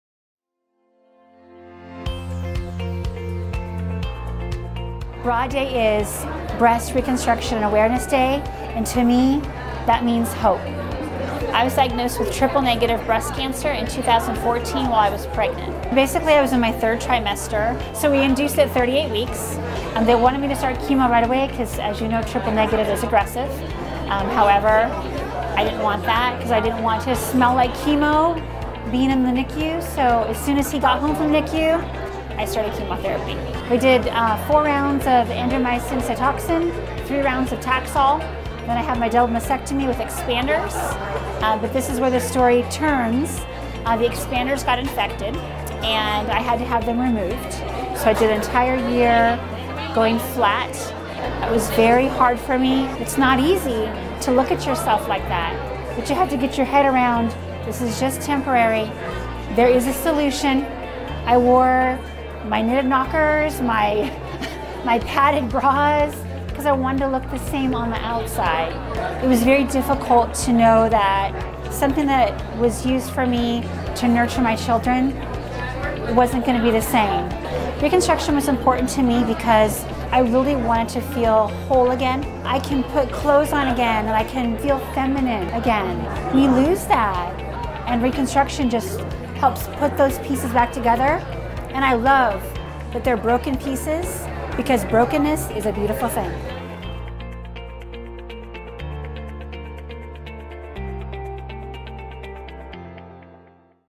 Breast Reconstruction Patient - Genny Small
Genny Small found out she had breast cancer when she was pregnant with her 8th child. After delivering her son early,  Genny started her journey with breast cancer treatment and eventually had DIEP Flap reconstruction surgery. Find out how it made Genny feel whole again.

Remember, early detection saves lives. Schedule your mammogram today.